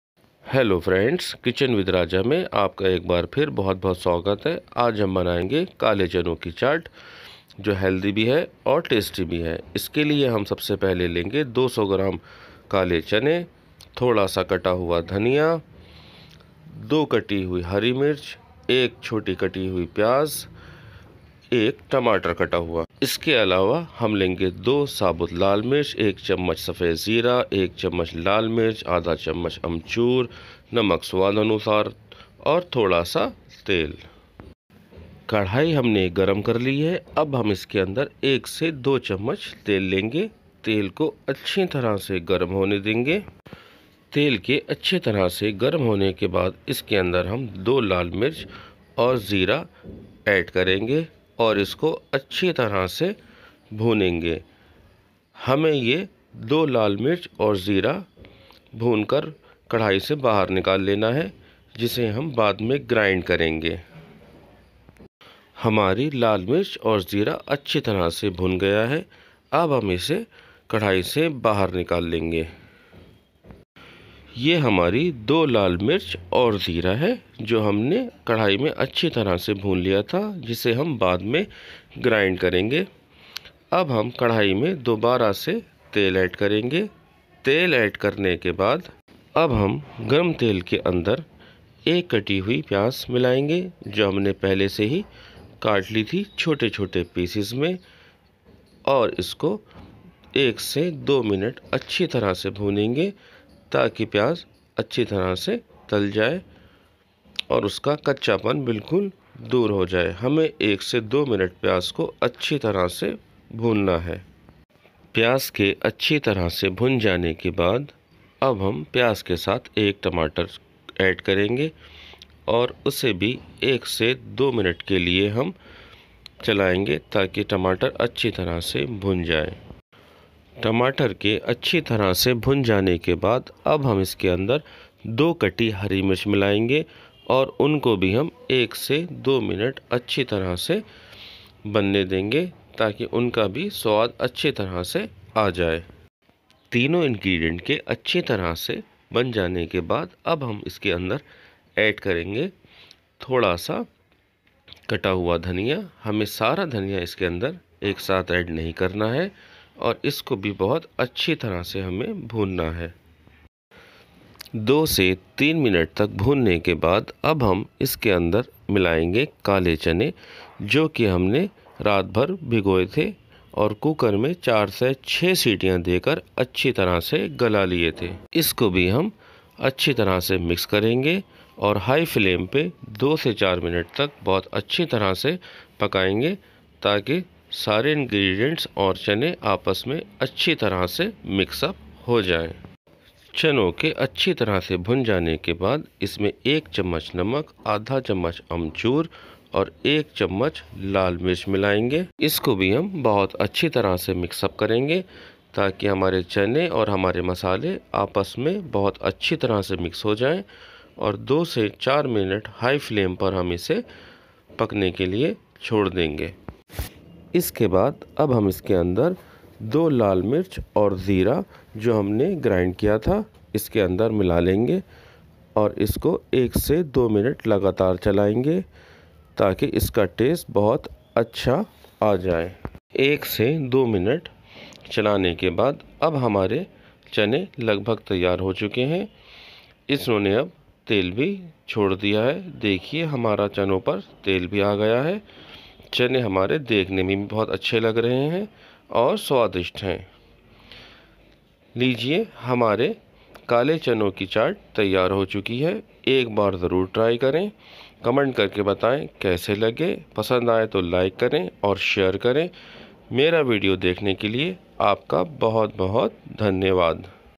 Kale chane ki chat.chaat recipe.sukhe chane ki chat.masala chana.kitchen with raja.काले चने की चाट।
In this recipe I try to make  delicious kala chana chaat. Plz try ones and share your experience.
Thanks
Kitchen with raja.